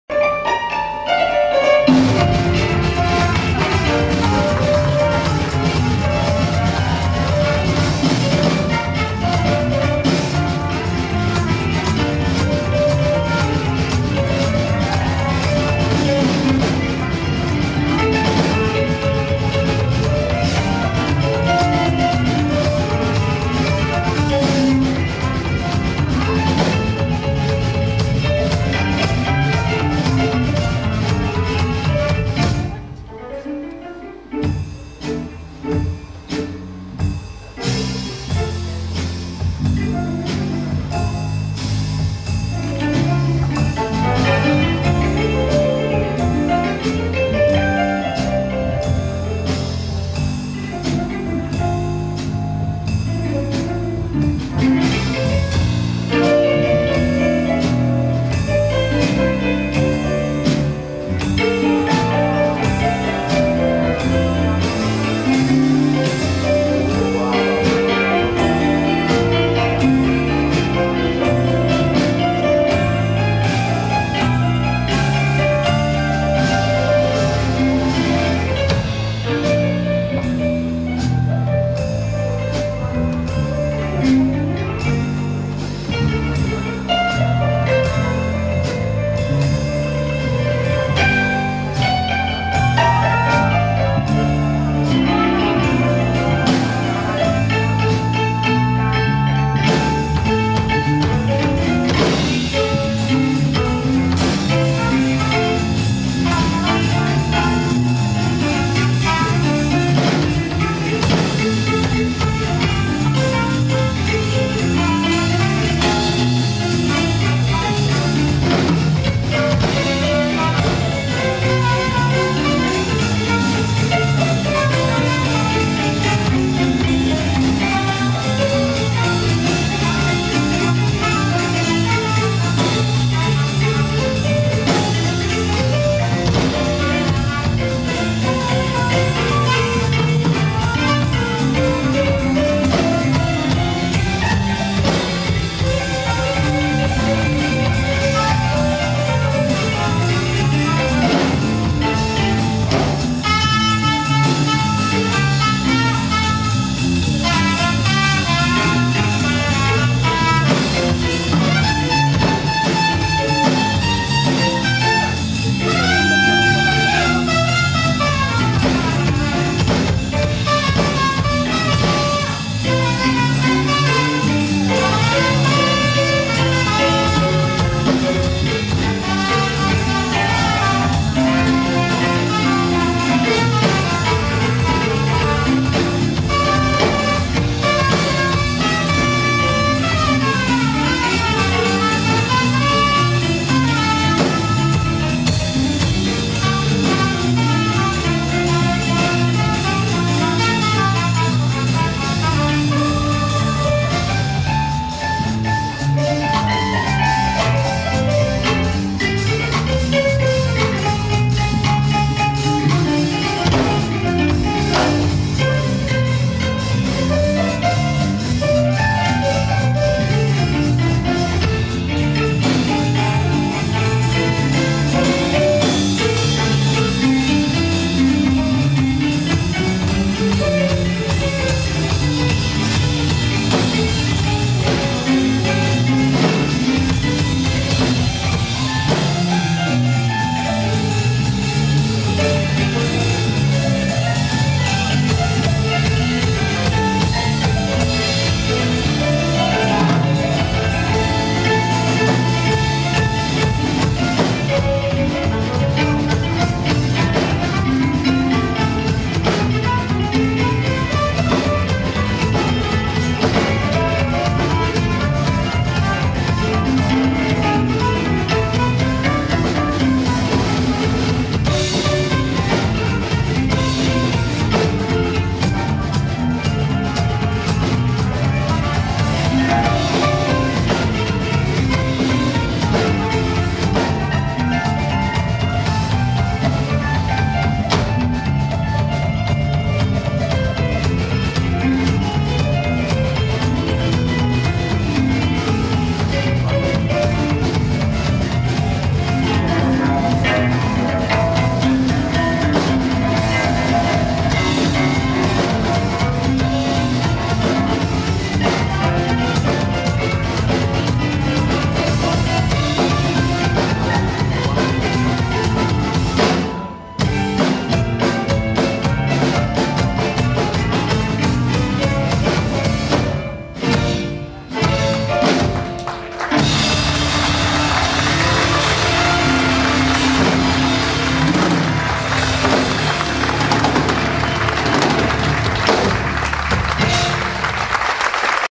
mihalache09
marius mihalache band in concert la festivalul zavaidoc pitesti 2008